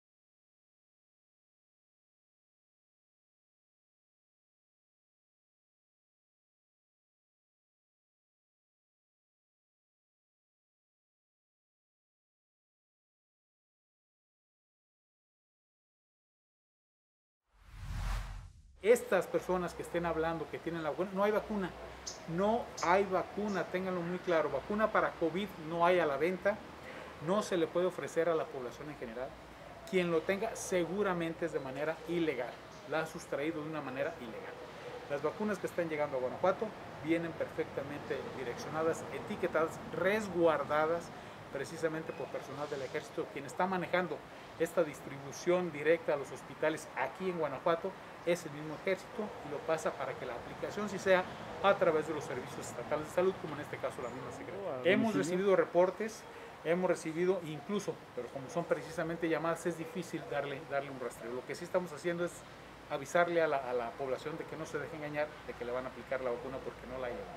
Alertan de falsa aplicación de vacunas para adultos mayores en Guanajuato
La Secretaría de Salud del Estado de Guanajuato alerta sobre supuestas llamadas para adultos mayores de que supuestamente les pondrán la vacuna anticovid.

I N S T A G R A M
F A C E B O O K ​​
T W I T T E R   Twitter.com/tv4guanajuato